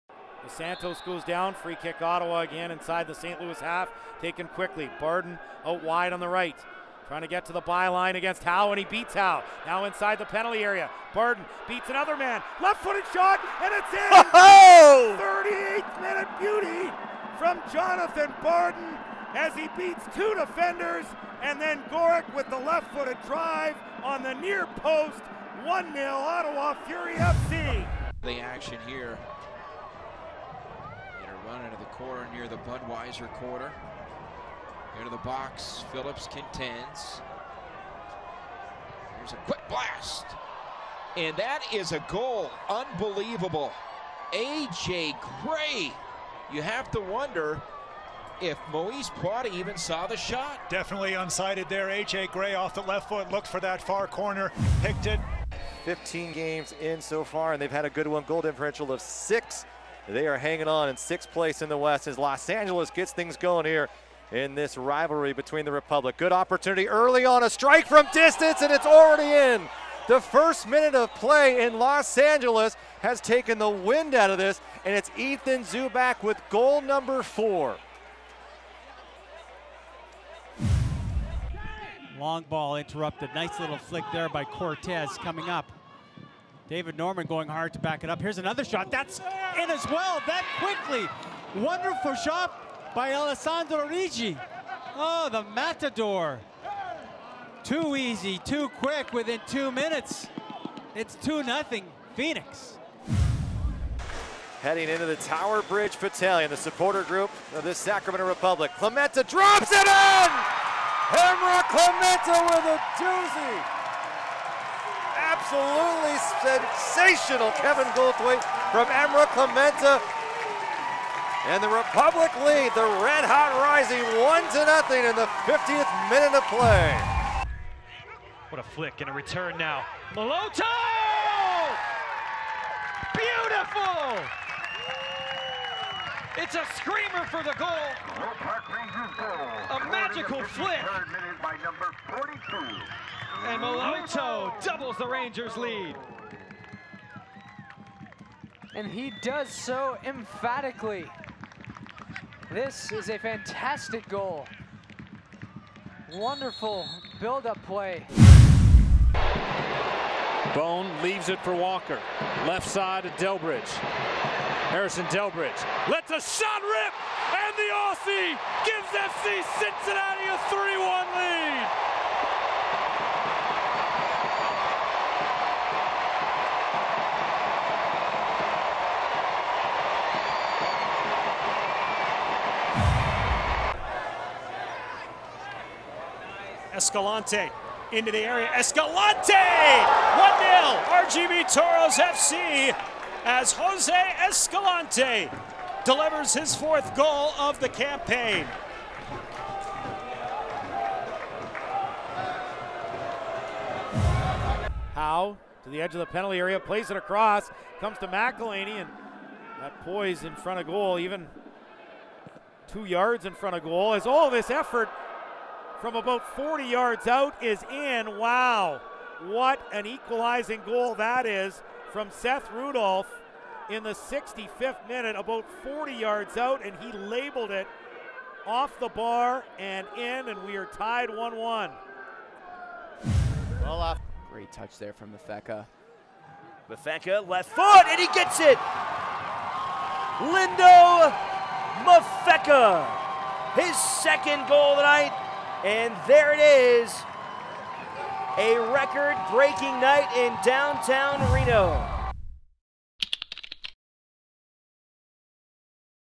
2017 USL Goal of the Month - September
Your chance to choose the best finish from the month of September in the USL.